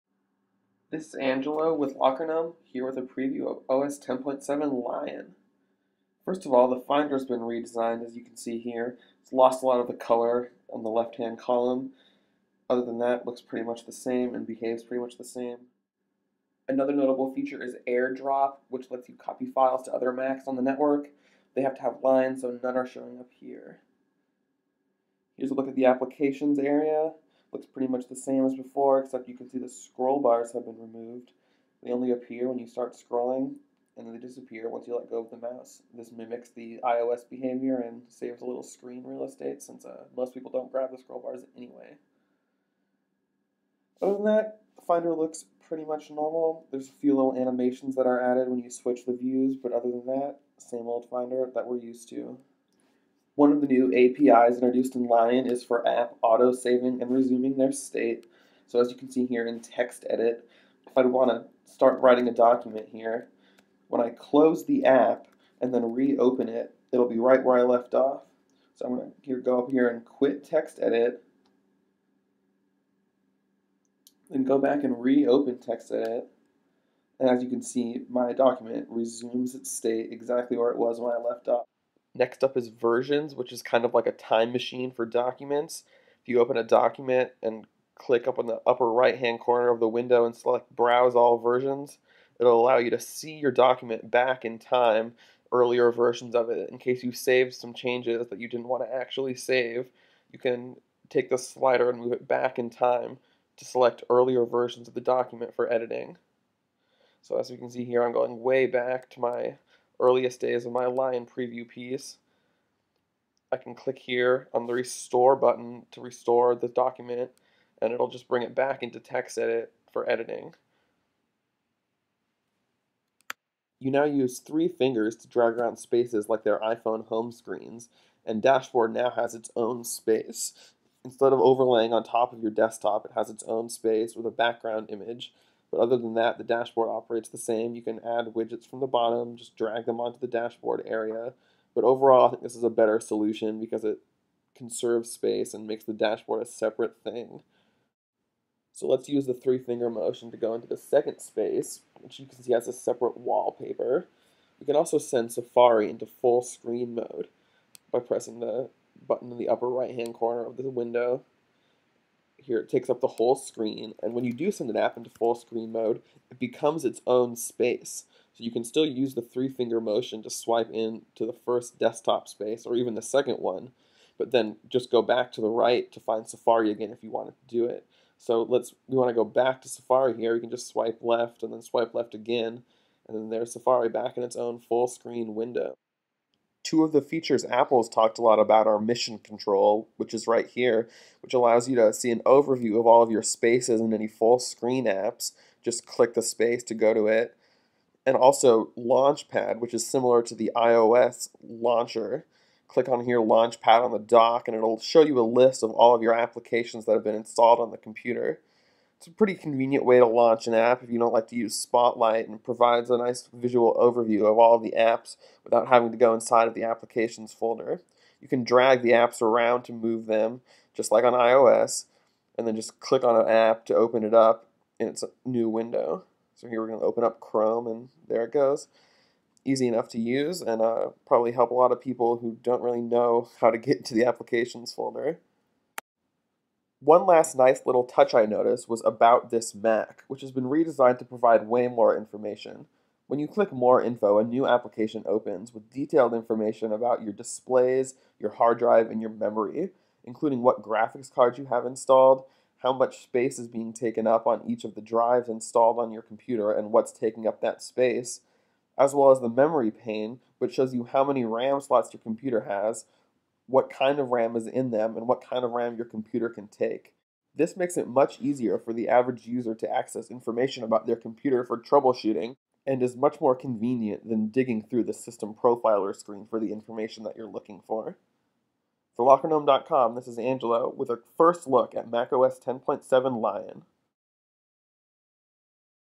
Mac OS 10.7 Lion Walkthrough Video
A quick walkthrough of some of the notable new features in Mac OS 10.7